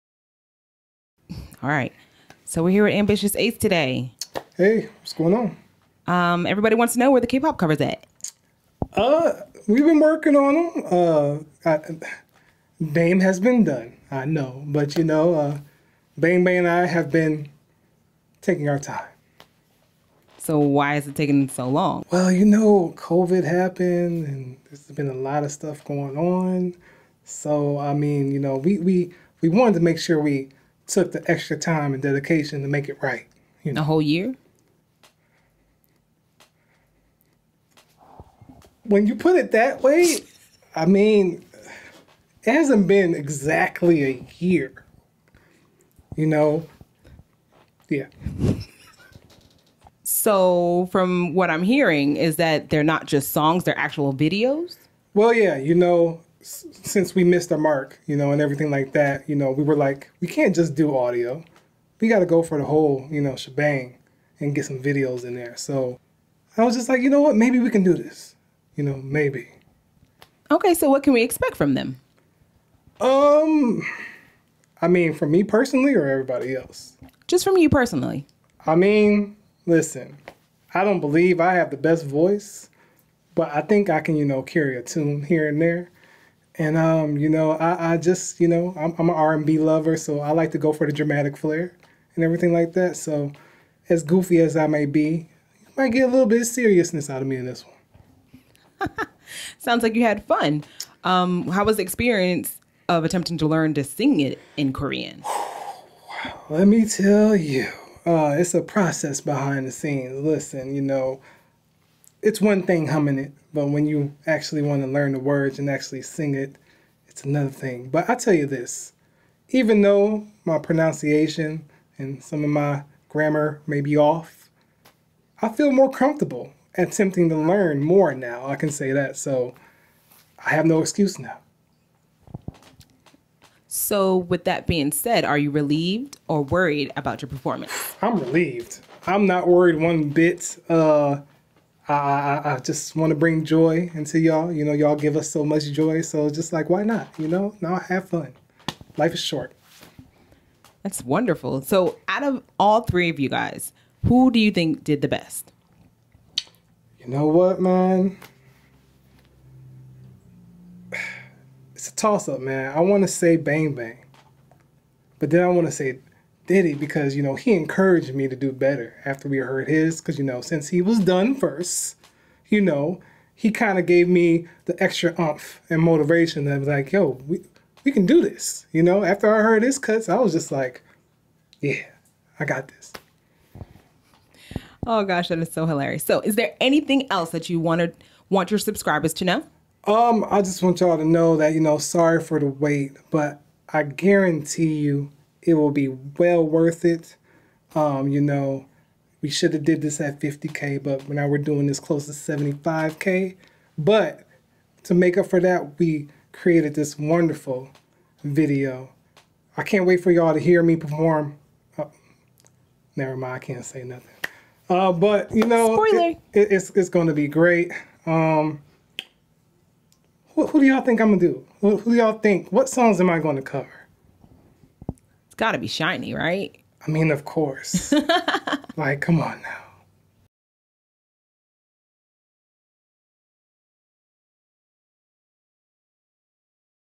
Where are the K-Pop Covers? Interview with Ambitious Ace
Ace has some explaining to do!  TRC was supposed to release K-Pop Covers after reaching 50,000 subscribers!  Where are they?

Grade Scale

Check

Dash

X